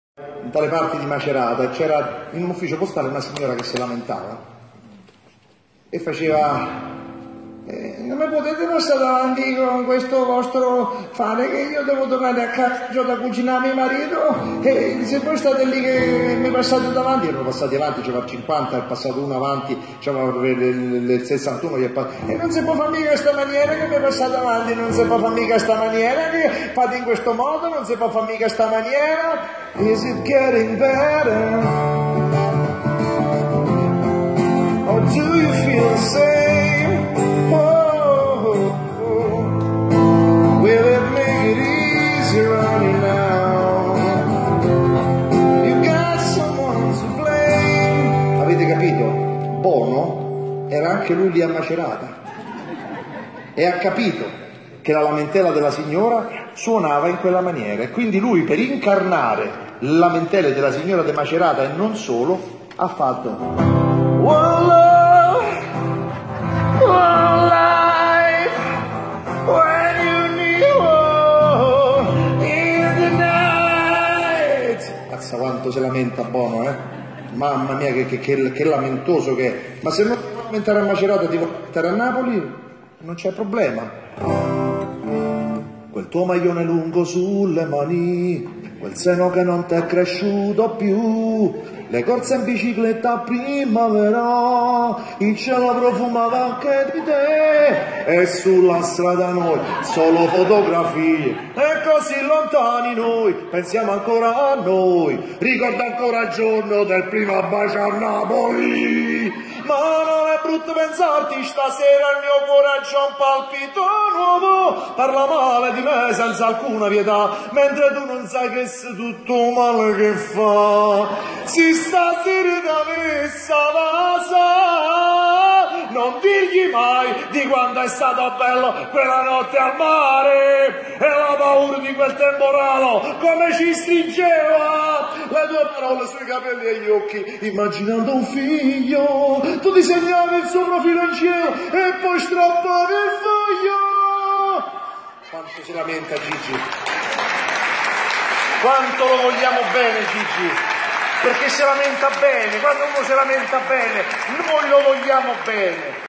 XV Edizione Festival d'arte Apuliae - spettacolo del 12 agosto 2018
Spettacolo SOLO PER VOI di e con Max Paiella. Foggia, Chiostro Santa Chiara, 12 agosto 2018.